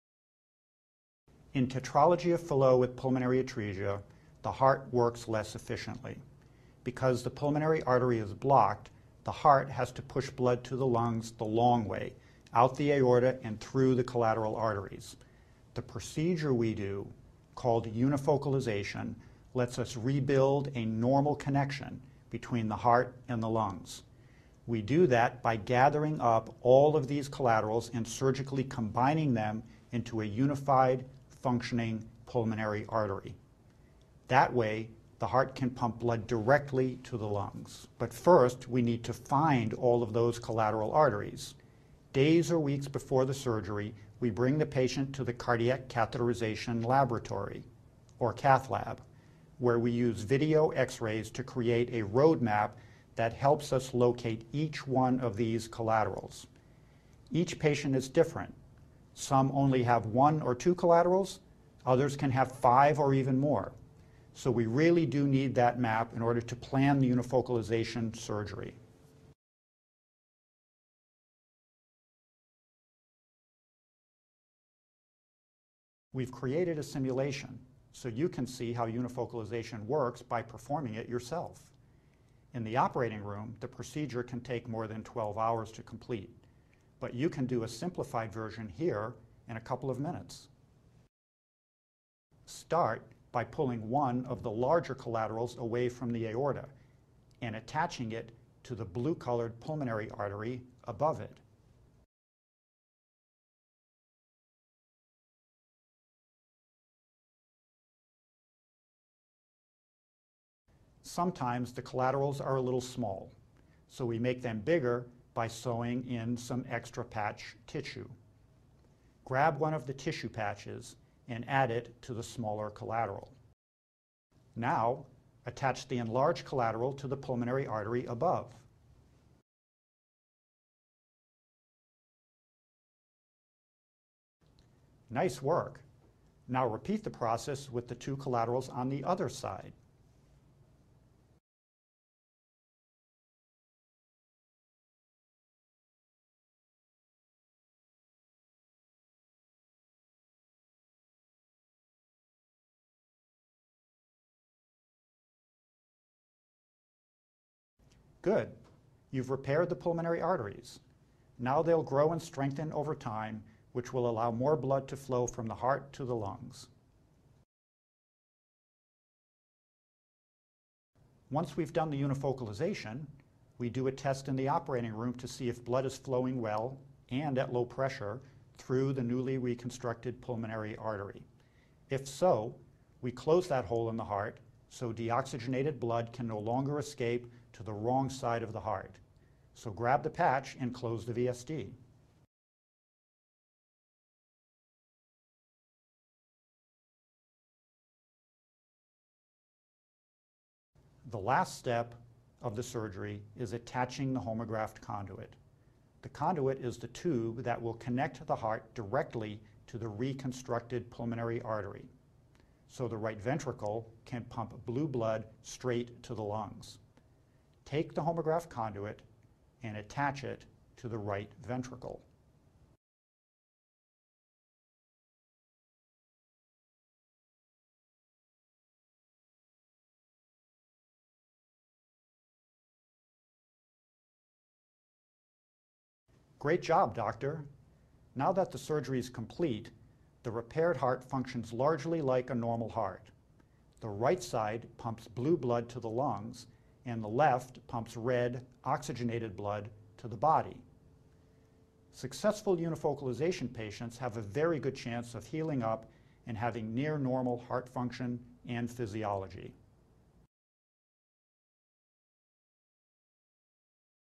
Chapter 3: Unifocalization & Repair - Stanford Children's Health
In this video, you will see a simulation of the unifocalization procedure, in which the surgeon rebuilds the blood vessel connection between the heart and lungs.